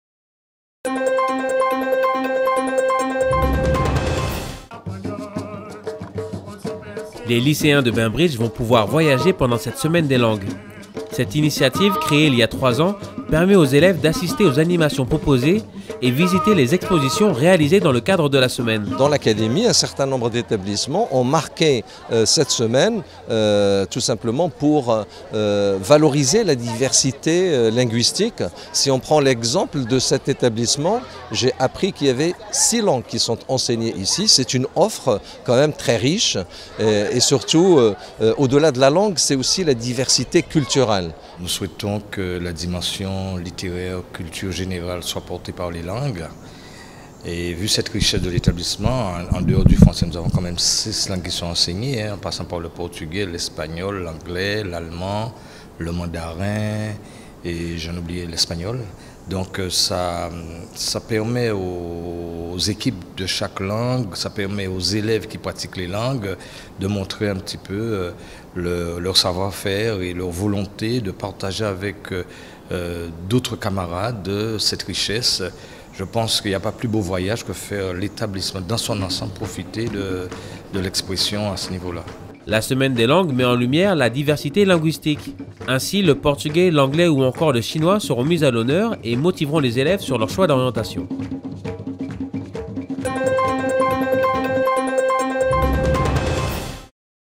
ETV NEWS - Semaine des langues
Les lycéens de Baimbridge vont pouvoir voyager pendant cette semaine des langues.
Cette initiative crée il y a 3 ans, permet aux élèves d’assister aux animations proposées par les élèves et visiter les expositions réalisées dans le cadre de la Semaine.